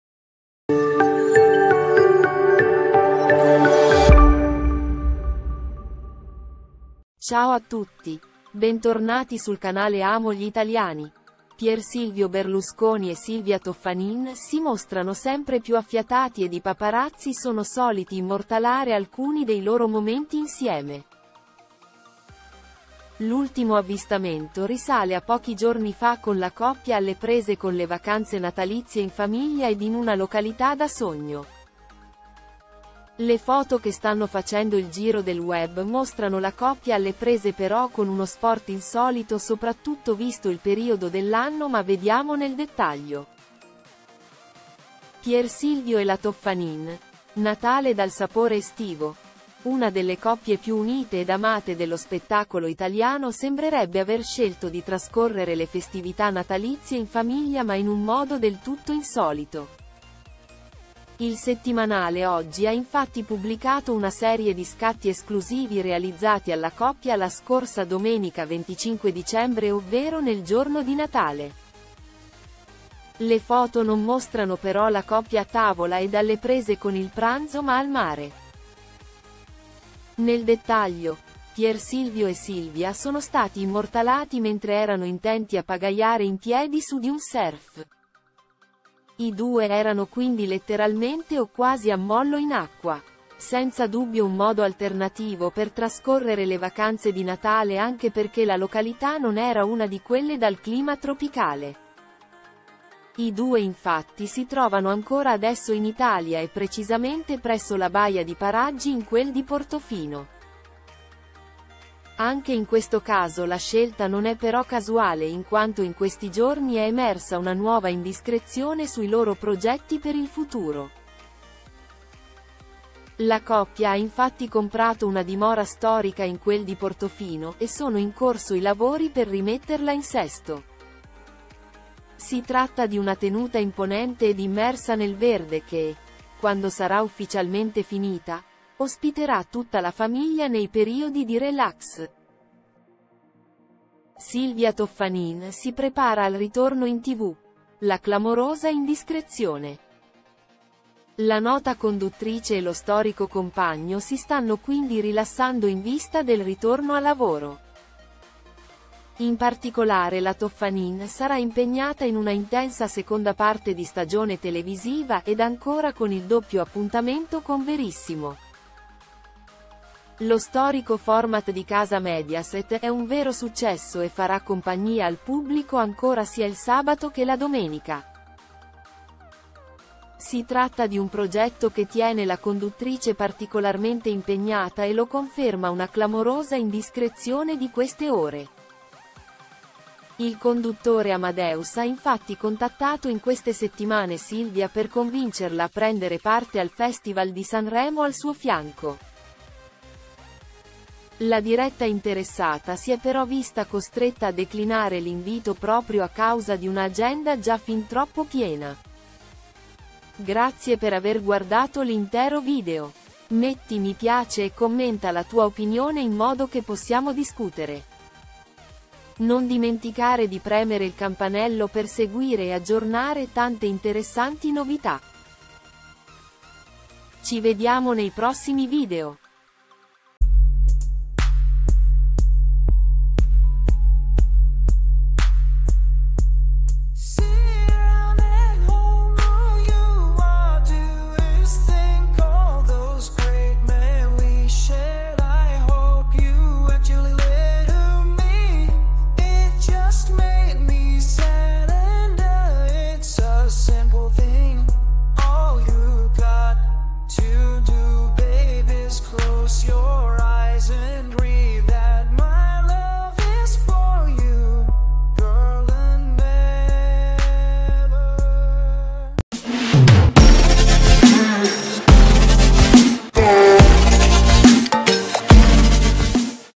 PierSilvio e Silvia Toffanin, inverno bollente per la coppia sono stati beccati così
PierSilvio Berlusconi e Silvia Toffanin si mostrano sempre più affiatati ed i paparazzi sono soliti immortalare alcuni dei loro momenti insieme. L’ultimo avvistamento risale a pochi giorni fa con la coppia alle prese con le vacanze natalizie in famiglia ed in una località da sogno.

Le foto che stanno facendo il giro del web mostrano la coppia alle prese però con uno sport insolito soprattutto visto il periodo dell’anno ma vediamo nel dettaglio.